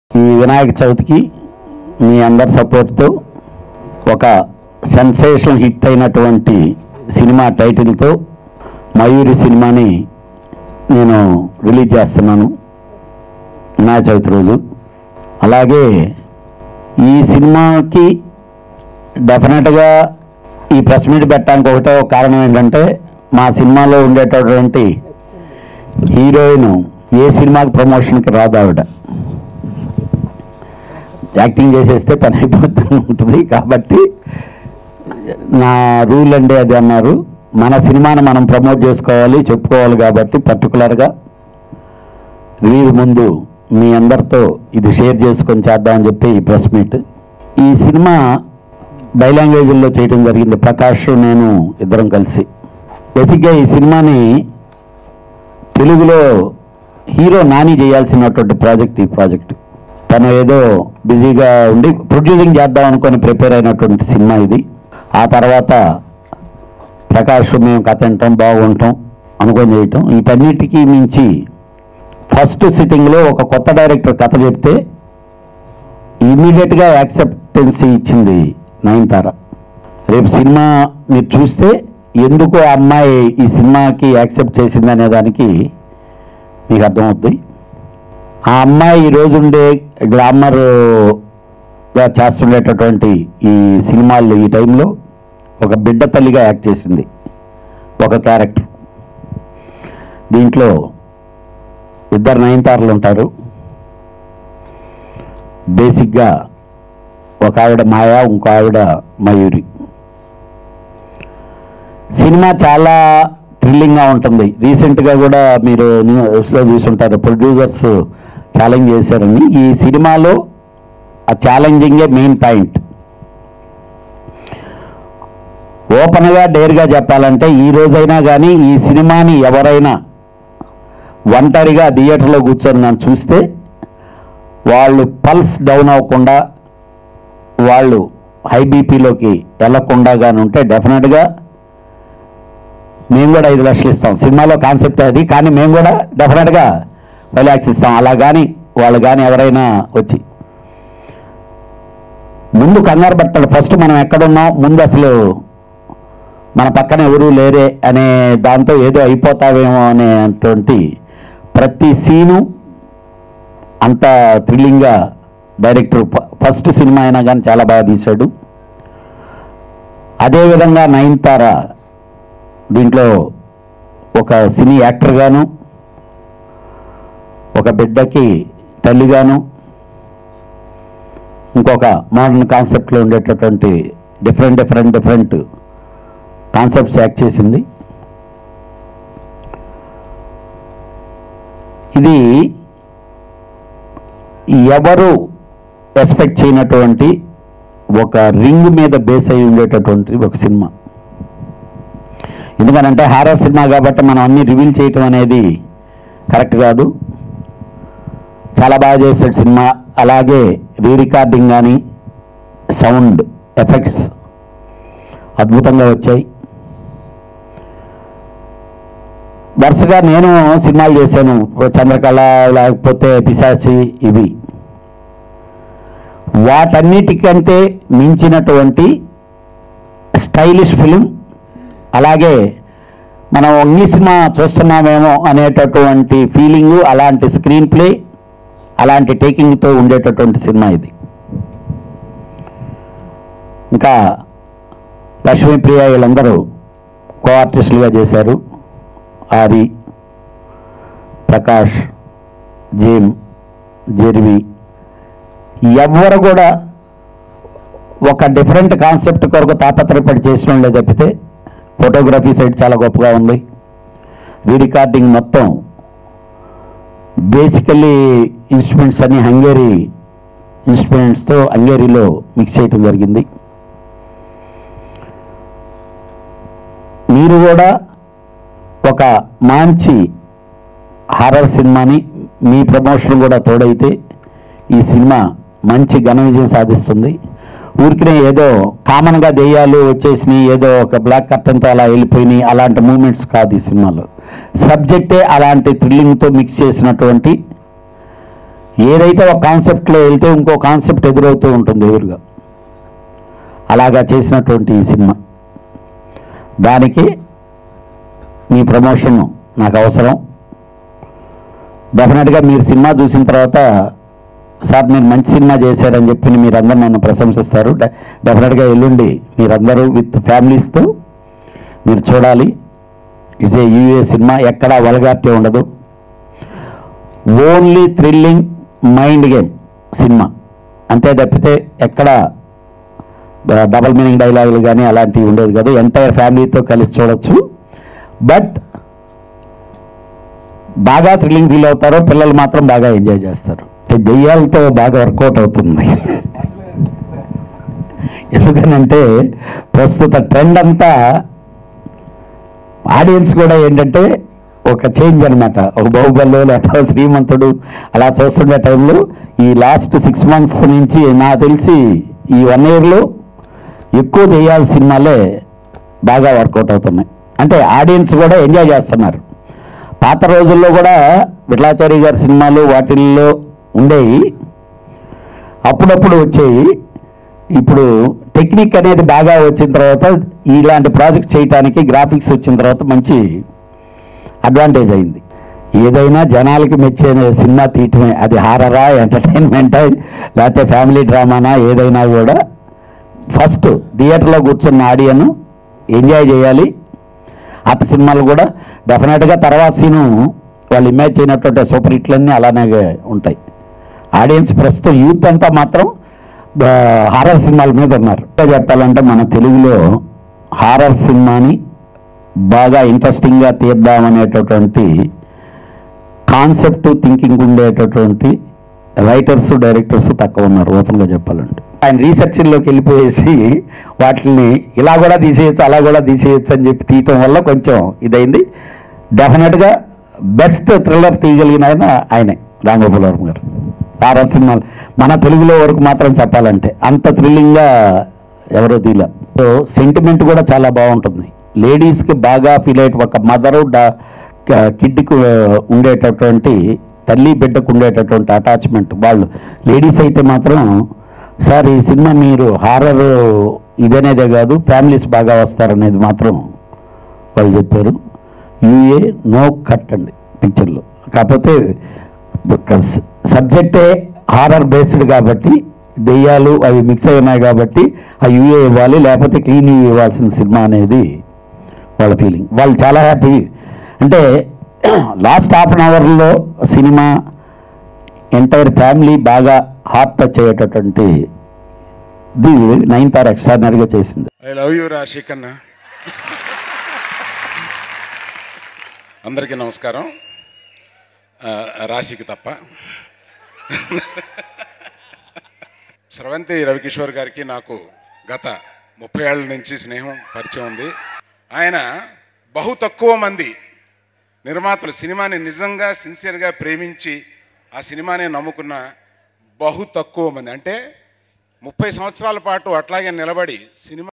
C kalyan Press Meet Mayuri Movie - Lollipop Cinema
Mayuri is an upcoming Indian supernatural horror film written and directed by Ashwin Saravanan. The film features Nayantara in the leading role, while Aari plays another pivotal role.